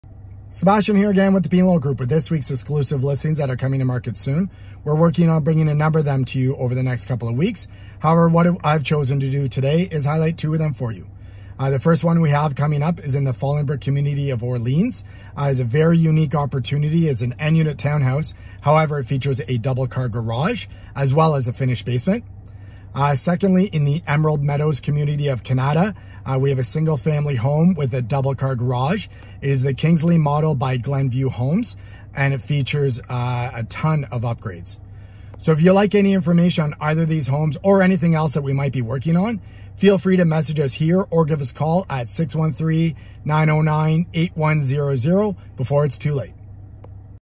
Exclusive | Coming Soon House Listings in Ottawa | Orleans | Fallingbrook | Kanata | Emerald Meadows
Are you having a hard time finding the perfect home? Check out our Exclusive Listings for this week that are coming to market soon in Ottawa and the surrounding area.